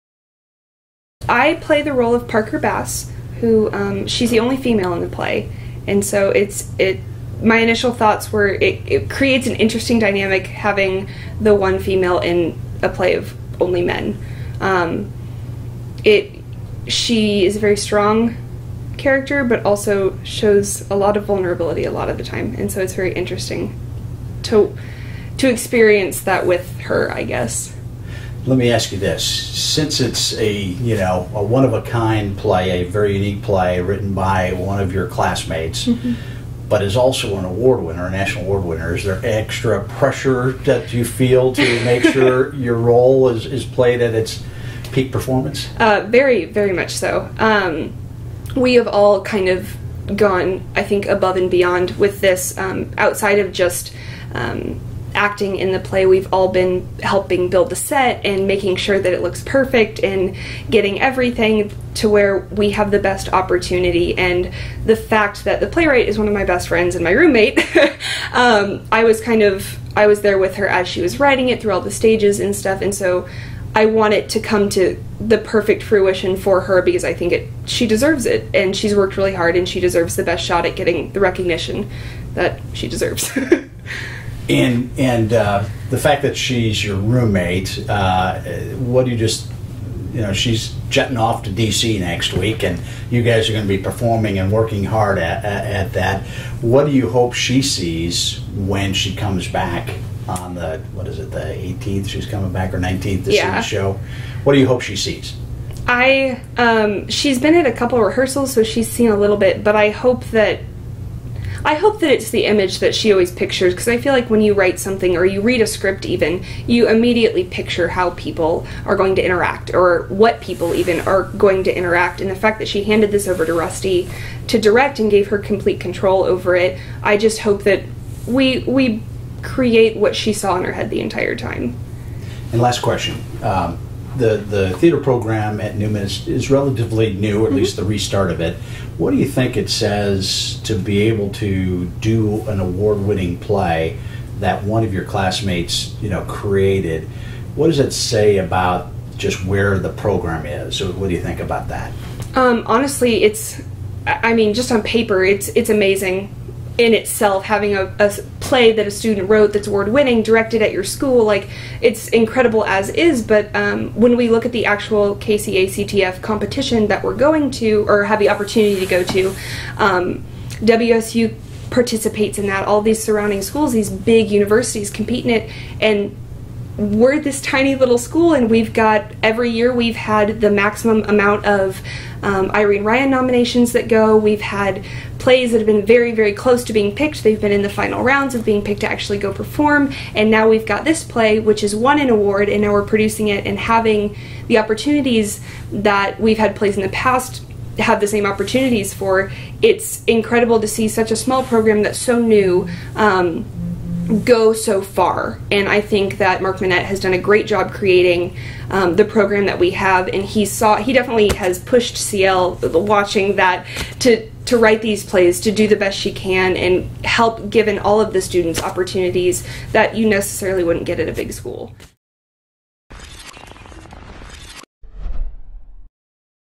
Newman University - Fair Departure - Original Play - Mary O'Neal
Newman University - Fair Departure - Original Play.  Actor Mary O'Neal, who plays Parker, talks about her role in the 2015 Kennedy Center American College Theater Festival award winning play and its debut performance April 16-19 in the Jabara Flexible Theatre, inside the De Mattias Fine Arts Center on the Newman Campus.